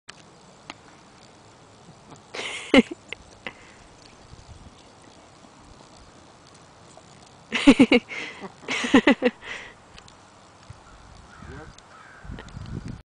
ShelbyGolf1.MPG
Shelby, our redbone coonhound, loves to be a golf ball bandit.